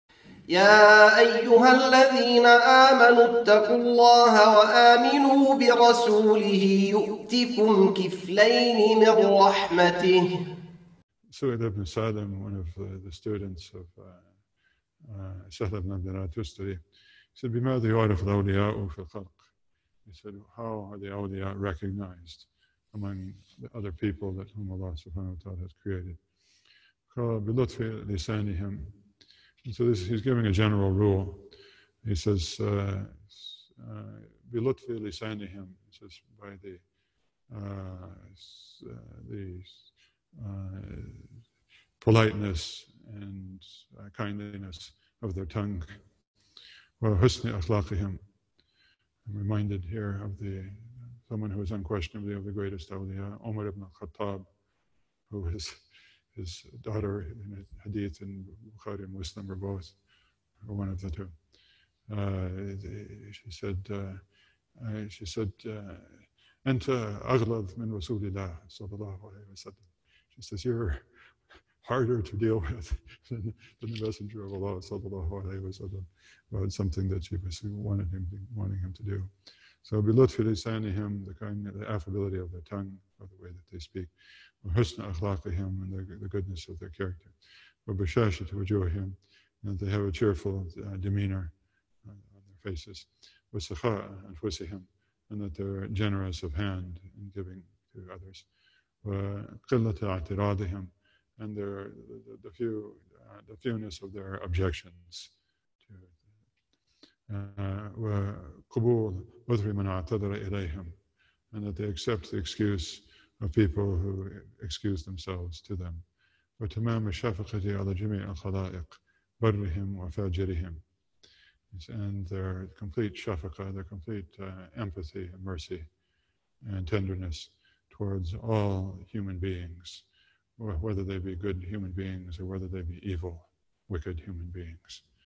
7 Traits _ How to Recognize a Saint
This is a video excerpt from a live class of the course "Gardens of the Righteous" – Sheikh Nuh Keller